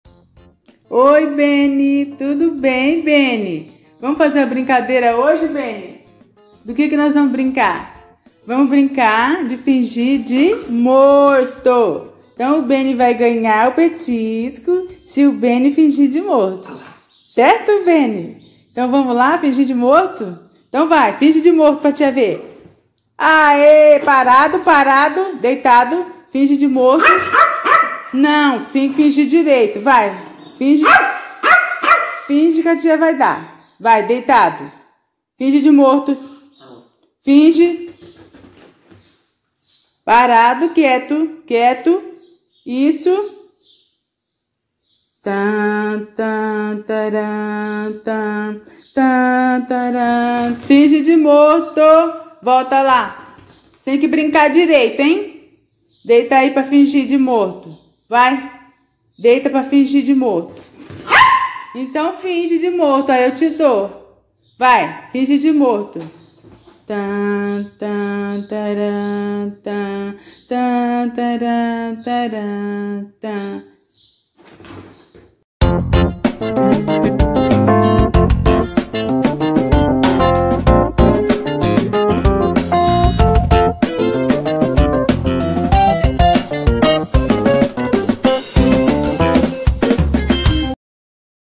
ESSE PINSCHER SABE SE FINGIR DE MORTO, VEM VER
NESSE  VÍDEO  FAÇO  UMA BRINCADEIRA  COM MEU CACHORRINHO,  ELE SE FINGE DE MORTO. 😘💋🙌🙏❤